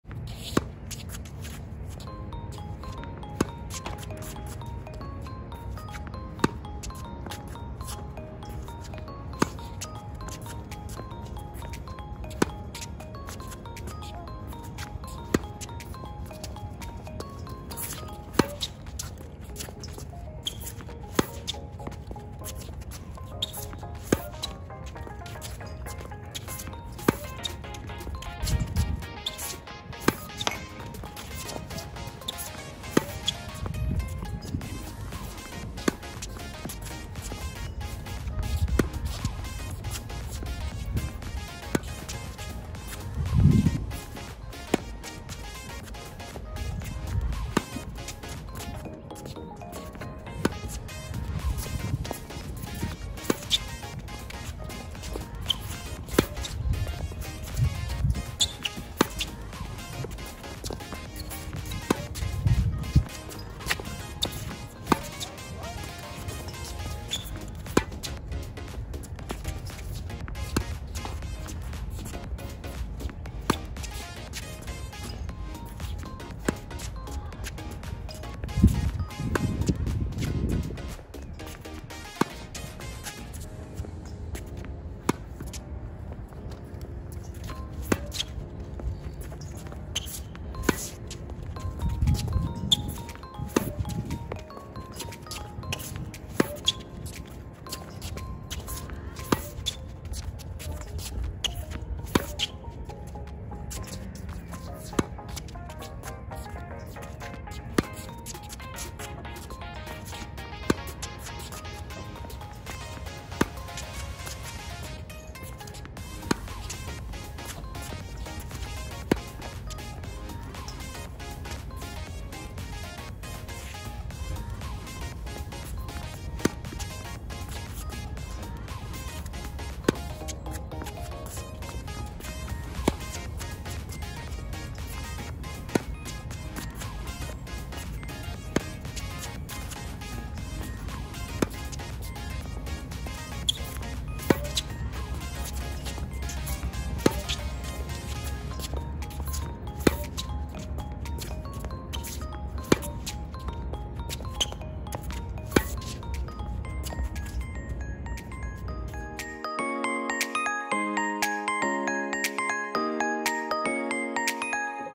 Morning Tennis Practice with Coach Slinger Bag / スリンガーバックを使って親子で朝練
Here is some forehand drill that we do in the morning using Slinger bag.

スリンガーバッグ (Slinger Bag)を使ったテニスの朝練の様子です。
スリンガーは疲れないので、いっぱいボールを打つことができます。

Appreciate great music by ikon. Here is his channel